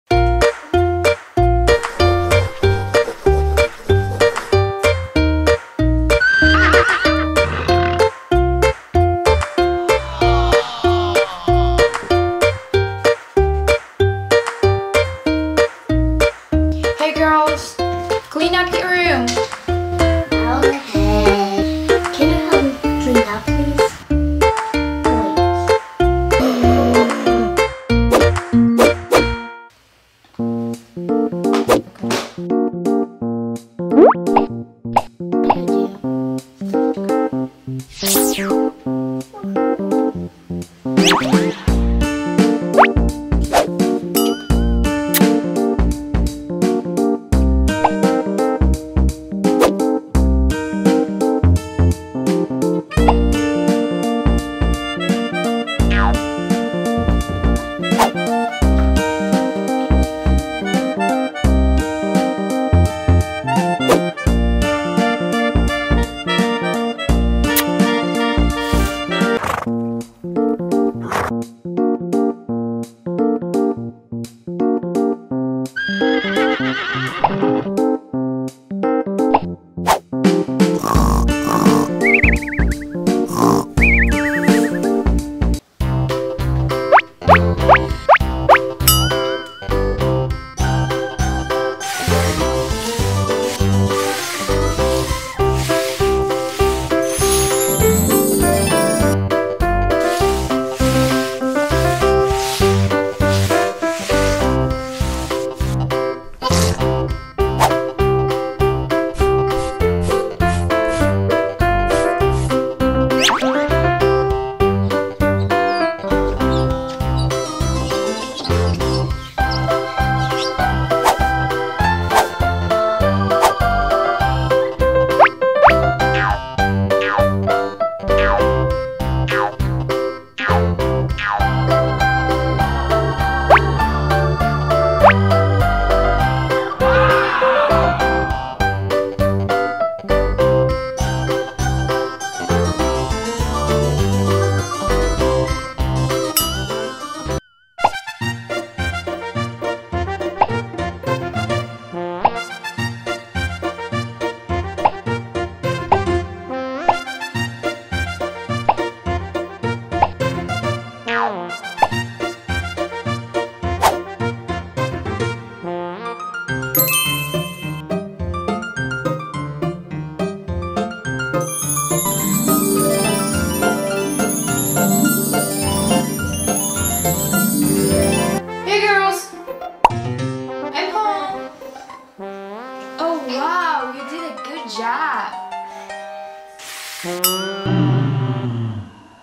Michelle pretend play with cleaning toys for kids.
Little girl Michelle plays cleaning with cleaning toys. She helps Mommy ! While little girl is cleaning the house her sister is sleeping. Michelle pretend play with Cleaning Toys! Funny kids video on kids patrol channel. Funny kids show with cleaning tools and mop on youtube.